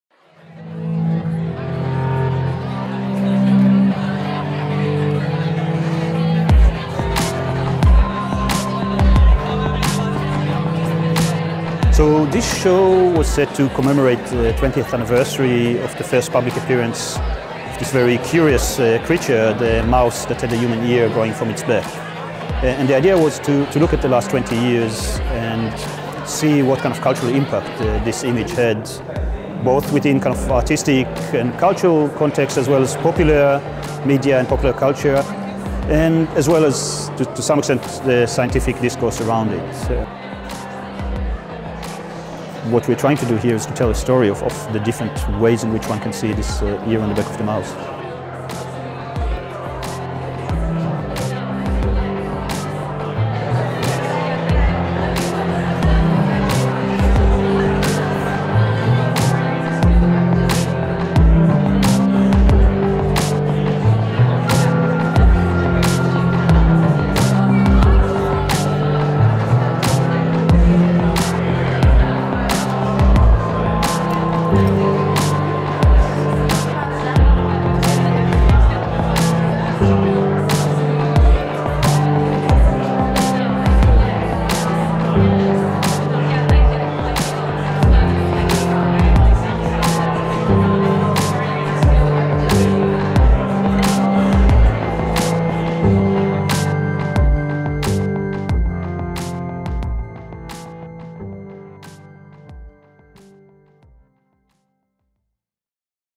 DeMonstrable - Exhibition Opening
One evening in October 1995, the BBC aired striking footage of a nude mouse with a human ear growing on its back. This evocative living object became one of the defining images of the late twentieth century, a symbol of our increasing capacity to shape and alter living bodies.

DeMonstrable is collaborative exhibition at the Lawrence Wilson Art Gallery, University of Western Australia - curated by SymbioticA Director Oron Catts with Associate Professor Jennifer Johung and Associate Professor Elizabeth Stephens. It is an event set to commemorate, respond to, and reflect on the multifaceted cultural and scientific impact of the Earmouse.

New work commissioned for this exhibition is presented together with 20 years of artistic, scientific and popular culture responses to the Earmouse.

The exhibition opening took place on Friday October 2, 2015.

This project has been supported by the Australian Government through the Australia Council, its arts funding and advisory body.